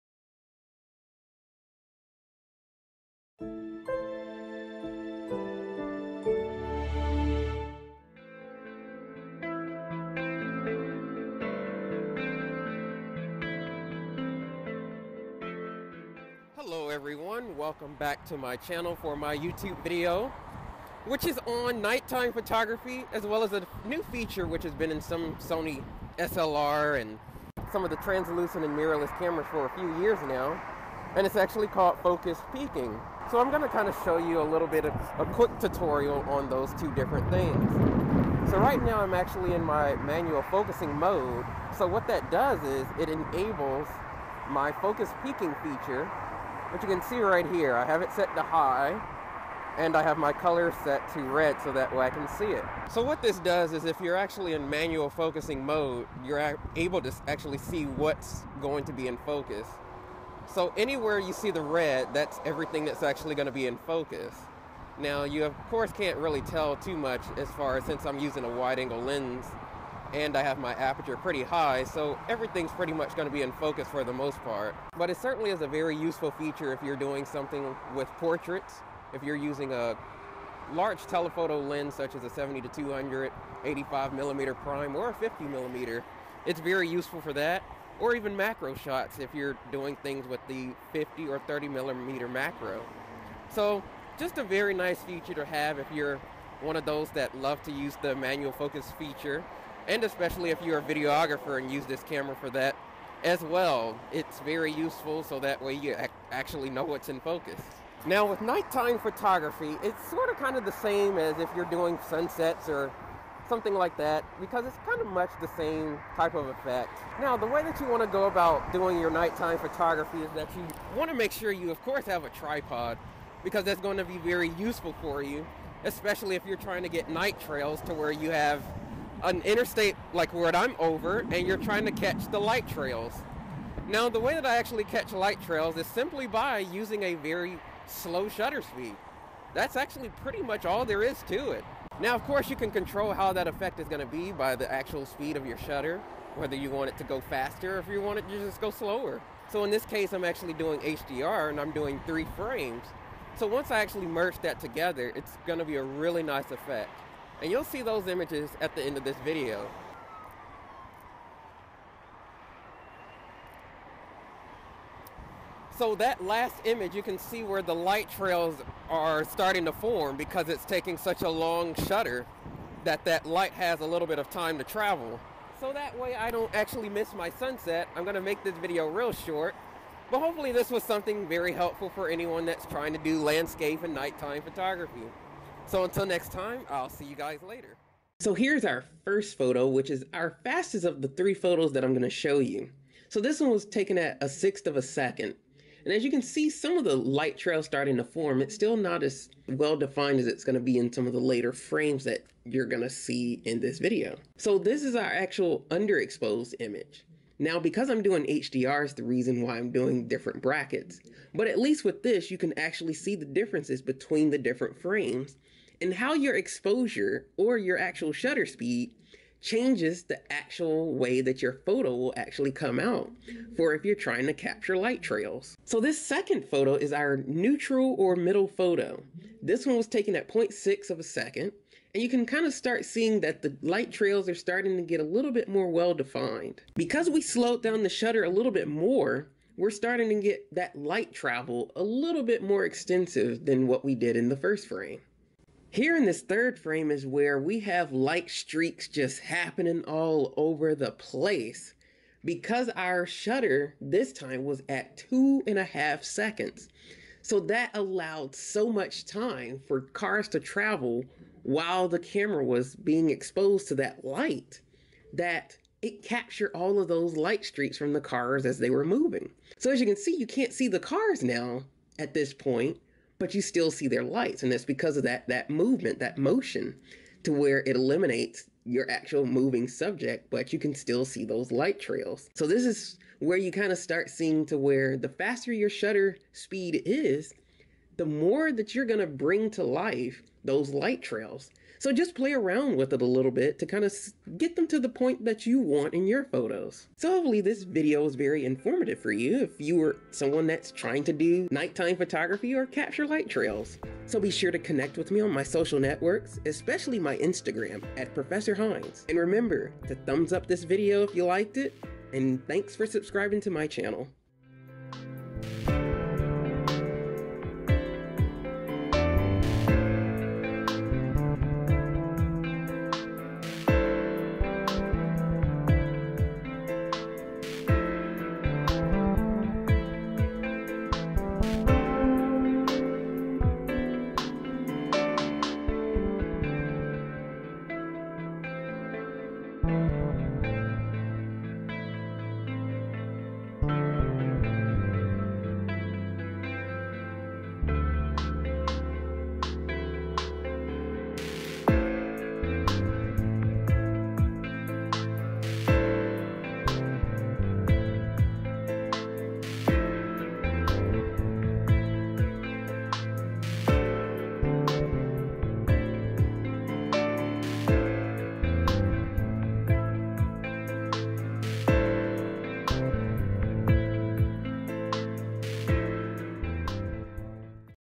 How-To: Photograph Light Trails - Night Photography & Focus Peaking on Sony a6000 - AskTheProfessor
for my full Photography, Tech, Beauty & Style blogs, tutorials and reviews.